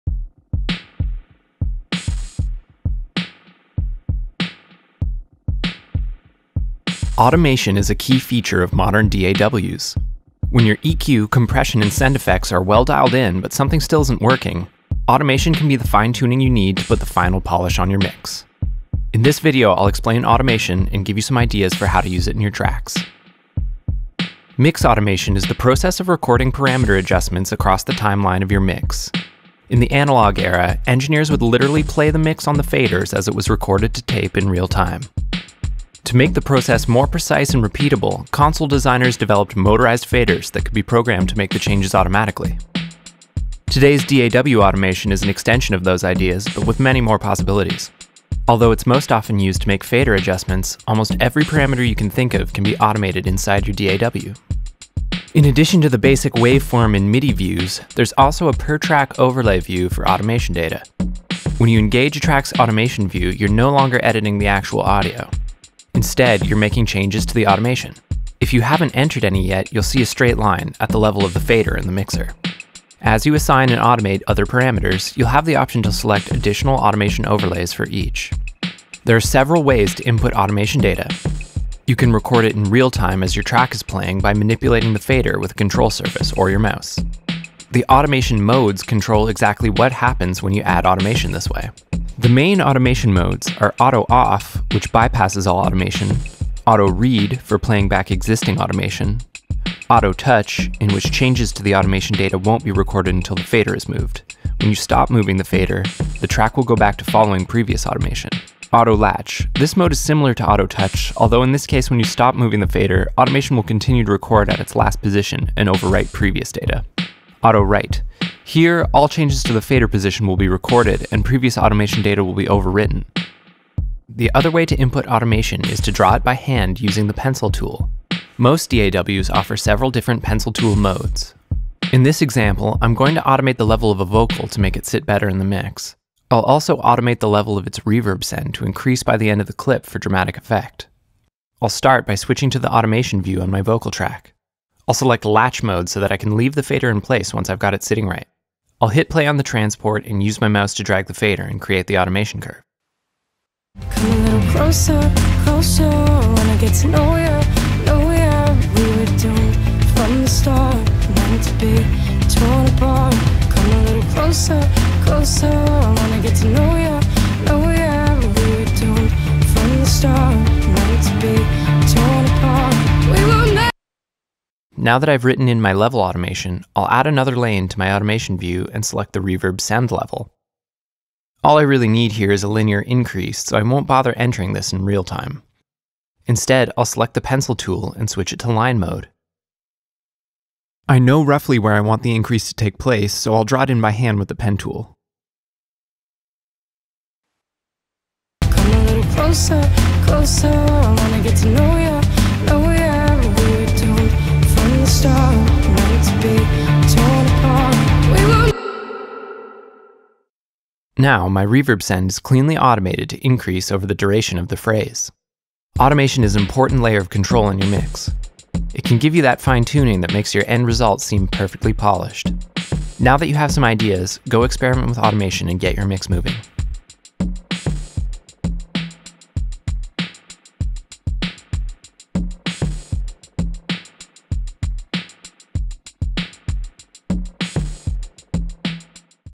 Mix Automation: How to Use Volume Automation and More | LANDR Mix Tips #5
Mix Automation is a key feature in most modern DAWs.

When your EQ, compression and send effects are well dialed in, but something still isn’t working—automation can be the fine-tuning you need to put the final polish on your mix.

In this video I’ll explain automation and give you some ideas for how to use it in your tracks.

Mix automation is the process of recording parameter adjustments across the timeline of your mix.

In the analog era, engineers would literally play the mix on the faders as it was recorded to tape in real time.

To make the process more precise and repeatable, console designers developed motorized faders that could be programmed to make the changes automatically.

Today’s DAW automation is an extension of those ideas, but with many more possibilities.

Although it’s most often used to make fader adjustments, almost every parameter you can think of can be automated inside your DAW.

In addition to the basic waveform and MIDI views on your DAW, there’s also a per track overlay view for automation data.

When you engage a track’s automation view, you’re no longer editing the actual audio. Instead, you’re making changes to the automation.

If you haven’t entered any yet, you’ll see a straight line at the level of the fader in the mixer.

As you assign and automate other parameters, you’ll have the option to select additional automation overlays for each.

There are several ways to input automation data.

You can record it in real time as your track is playing by manipulating the fader with a control surface or your mouse.

Your DAW’s automation modes control exactly what happens when you add automation this way.

The main automation modes are:

Auto Off: Bypasses all automation.
Auto Read: For playing back existing automation.
Auto Touch:  In which changes to the automation data won’t be recorded until the fader is moved. When you stop moving the fader, the track will go back to following previous automation.
Auto Latch: This mode is Similar to Auto Touch, although in this case when you stop moving, the fader automation will continue to record at its last position and overwrite previous auto.
Auto Write: Here, all changes to the fader position will be recorded and previous automation data will be overwritten.

The other way to input automation is to draw it by hand using your DAW’s pencil tool. Most DAWs offer several different pencil tool modes.

In this example, I’m going to automate the level of a vocal to make it sit better in the mix. I’ll also automate its reverb send to increase by the end of the clip for dramatic effect.

I’ll start by switching to the automation overlay on my vocal track. I’ll select “latch” mode so that I can leave the fader in place when I’ve got it sitting right.

I’ll hit play on the transport and use my mouse to drag the fader and create the automation curve.

Now that I’ve written in my level automation, I’ll add another lane to my automation overlay and select the reverb send level.

I know that all I need here is a linear increase so I won’t bother entering this in real-time. Instead I’ll select the pen tool and switch it to line mode.

I know roughly where I want the increase to take place, so I’ll draw it in by hand with the pen tool.

Automation is an important layer of control in your mix. It can give you the kind of fine tuning that makes your end result seem perfectly polished.

Now that you have some ideas, go experiment with automation and get your mix moving.

Read more about how to use automation to improve your mix

Follow the LANDR blog and get the best tips, tools and tutorials for producers